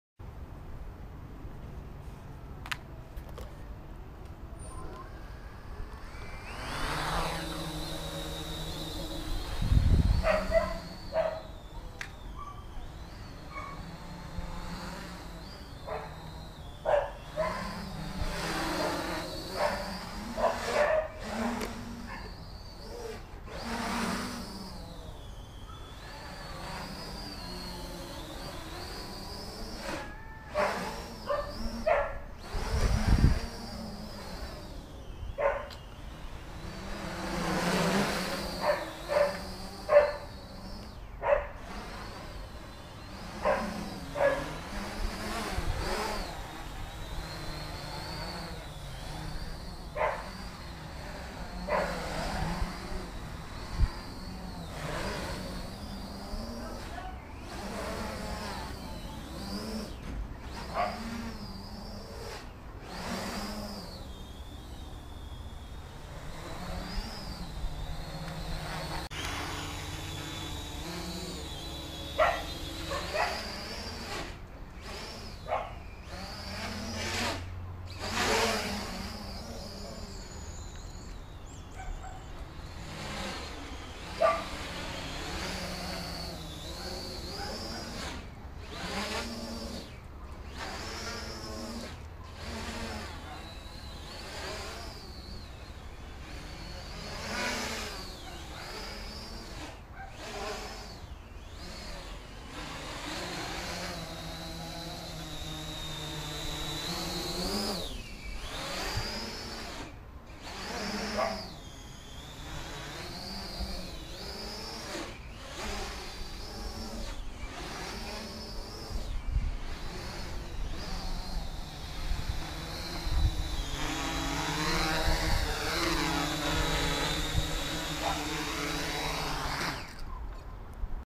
Karbonic-X flipping out
Had to get it out of my system after watching Warthox flip his around today.

KK V2 LCD  flight controller